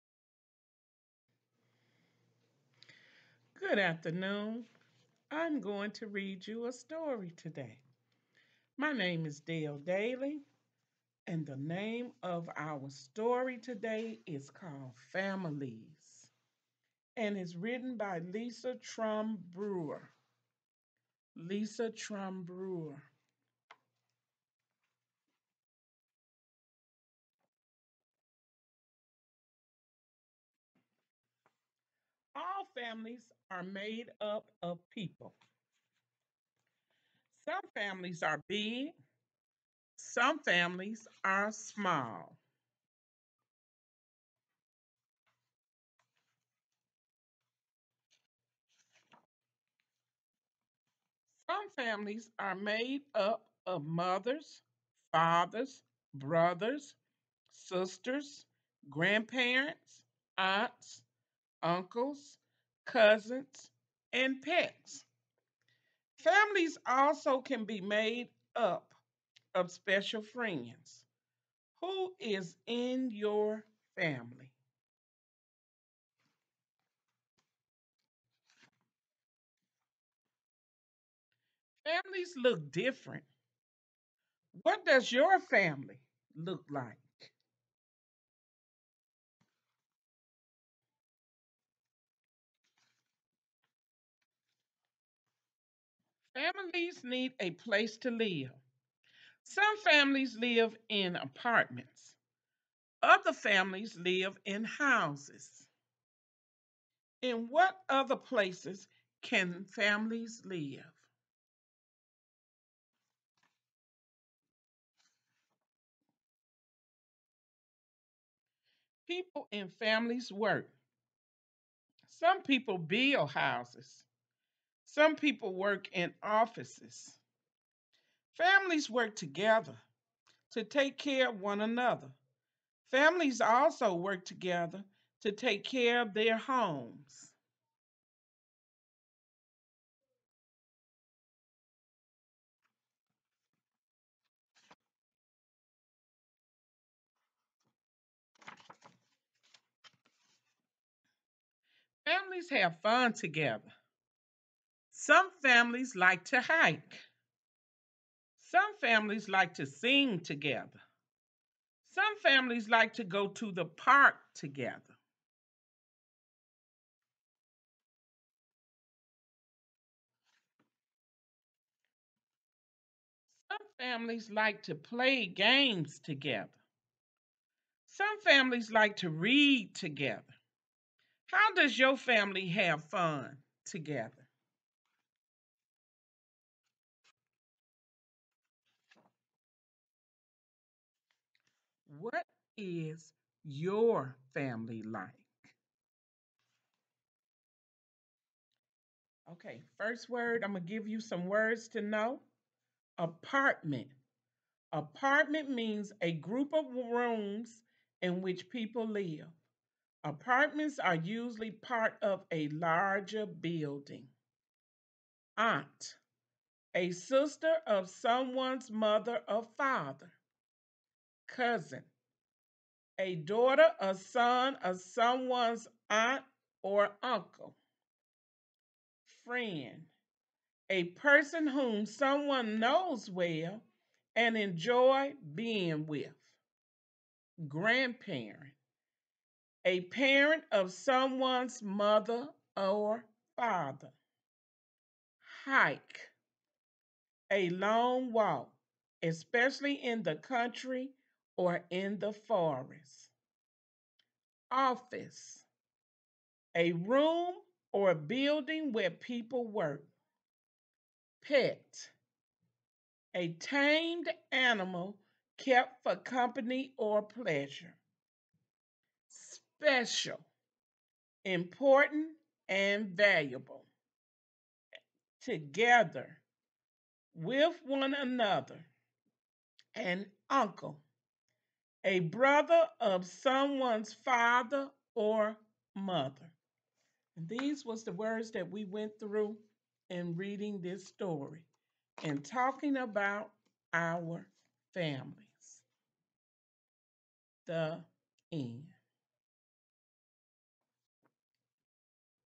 Families
By Lisa Trumbauer
Read aloud by Dale Dailey